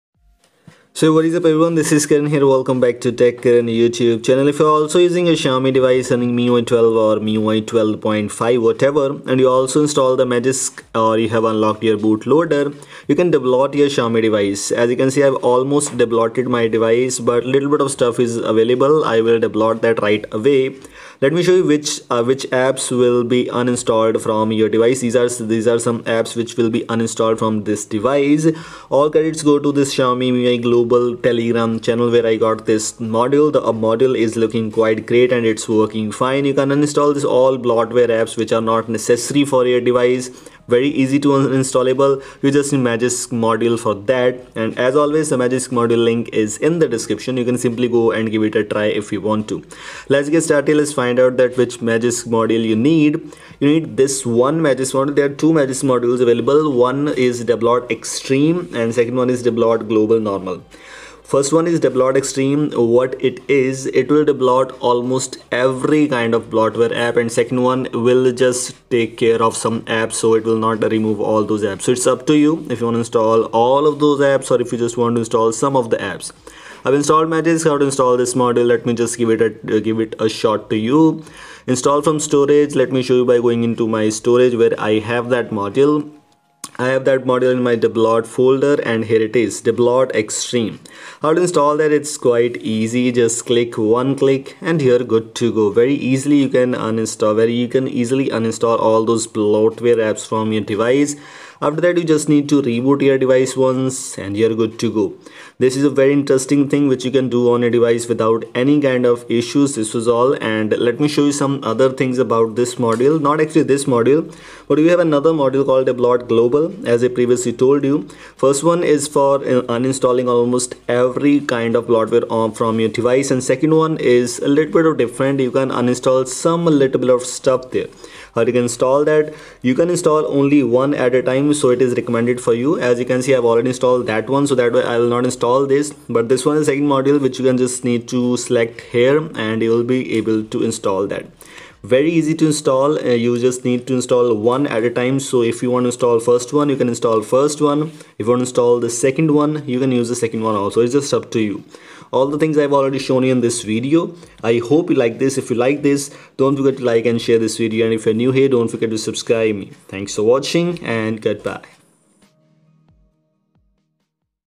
MIUI 12/12.5 - Easy Debloat your Xiaomi Device 🔥🔥 | Uninstall System Apps [No PC] [Root & Magisk]
Hey guys, What's Up? Everything good I Hope. In this Video, I'm gonna show you "MIUI 12/12.5 - Easy Debloat your Xiaomi Device 🔥🔥 | Uninstall System Apps [No PC] [Root & Magisk]" . Make Sure to watch this video till the end to understand everything.